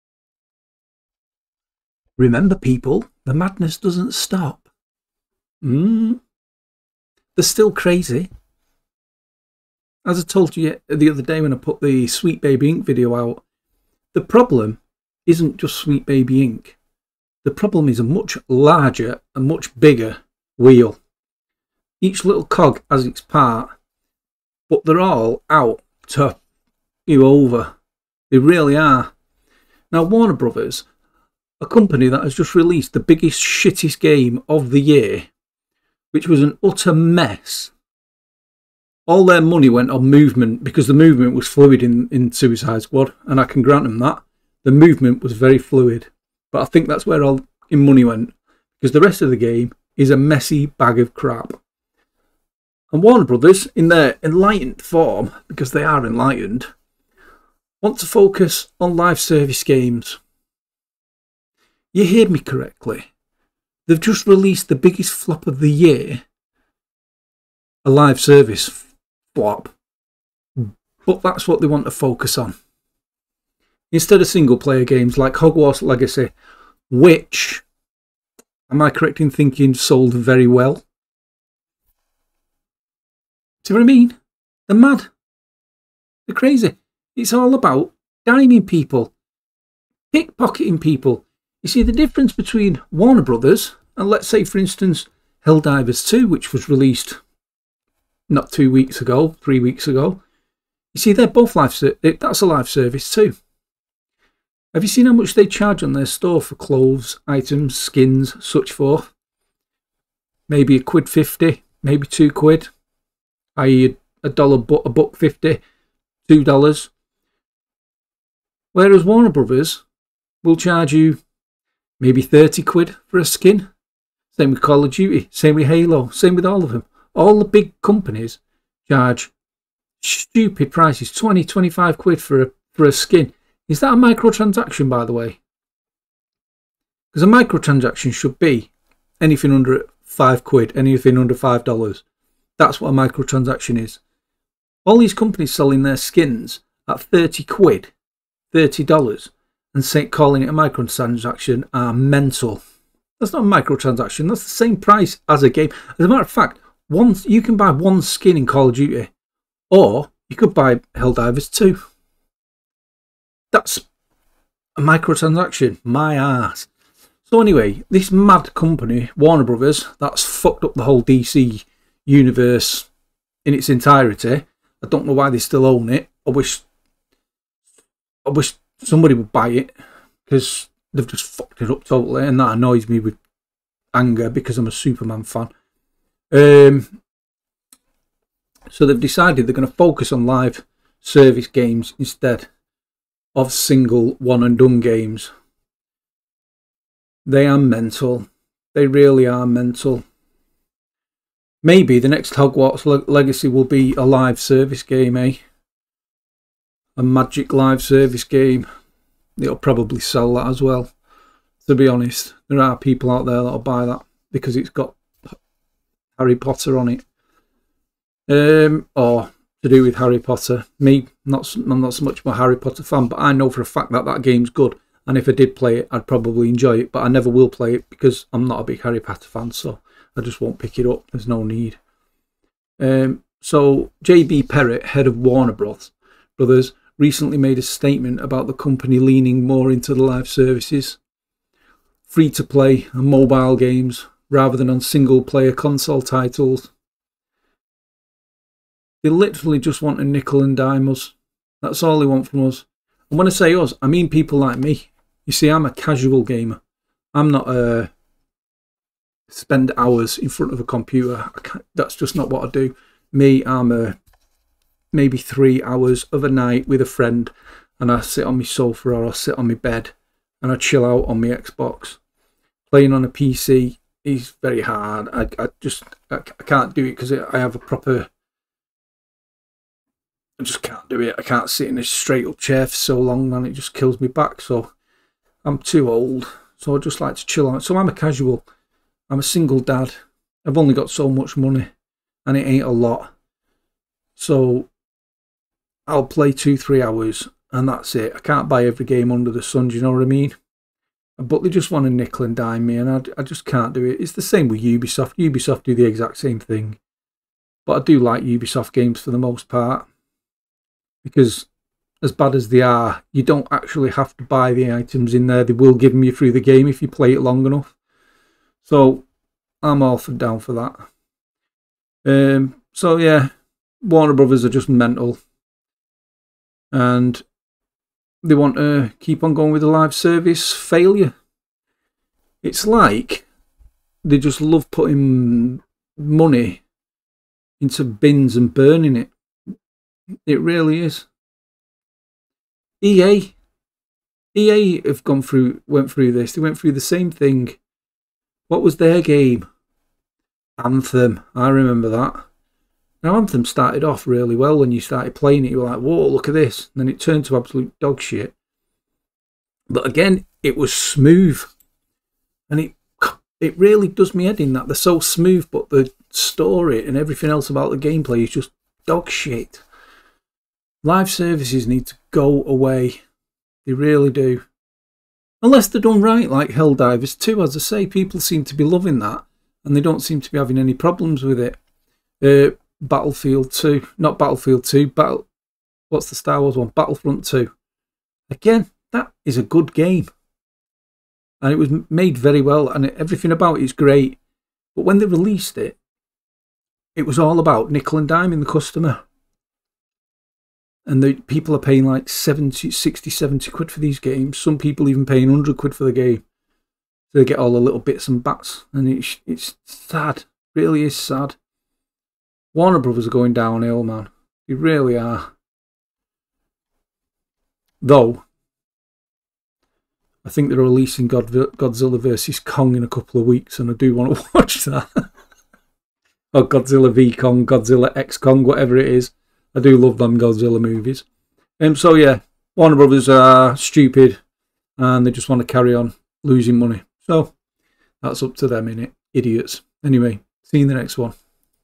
WARNER BROS... LIVE SERVICE Is The Way To Go really?
Warner Bros. is shifting its gaming focus to live service, free-to-play, and mobile games, potentially impacting future single-player titles like Hogwarts Legacy's sequel.
 The record-breaking success of Hogwarts Legacy is not deterring Warner Bros. from moving away from single-player console games.
 It's possible there may be live-service elements in Hogwarts Legacy 2 as Warner Bros. tries to replicate the first game's success. The last year has seen legendary highs and lows for the Warner Bros. gaming division. This February saw the release of Suicide Squad: Kill the Justice League, which did not meet critical or commercial expectations, with many criticizing its lackluster progression system, questionable combat framework, and ill-fitting live service implementation. On the other side of the spectrum, Hogwarts Legacy's February 2023 launch, as well as its follow-up release on last-gen consoles later in the year, proved to be a massive success for the company, with the game ultimately outselling every other 2023 release.